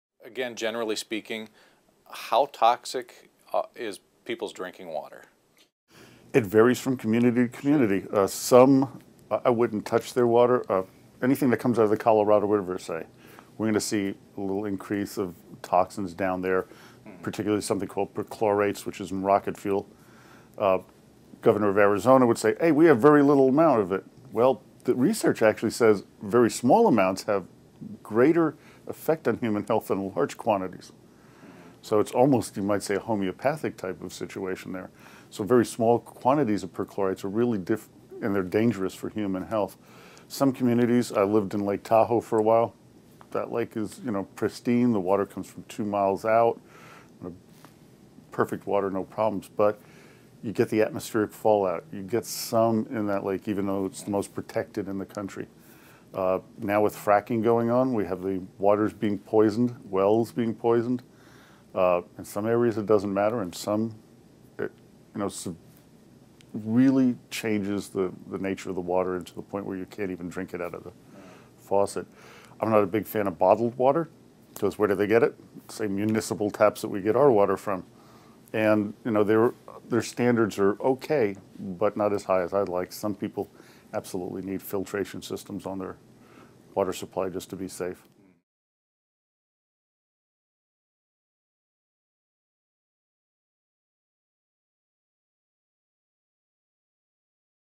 How Toxic Is Your Water?
Mark Schauss discusses potential toxins in your water supply.  How do you know what might be in there?  Is bottled water a safer option?  Find out what he thinks.